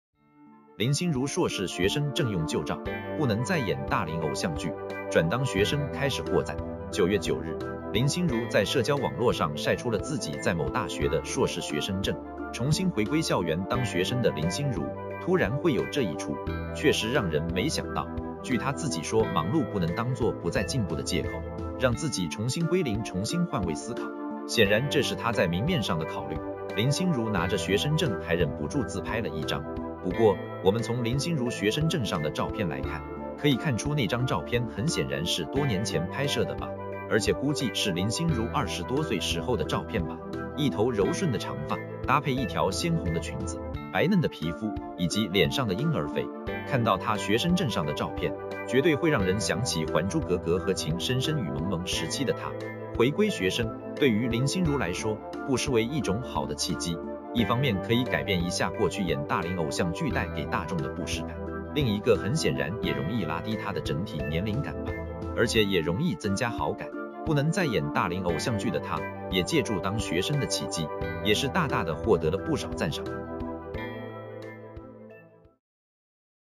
林心如硕士学生证用旧照,不能再演大龄偶像剧,转当学生开始获赞
➤Thanks收看！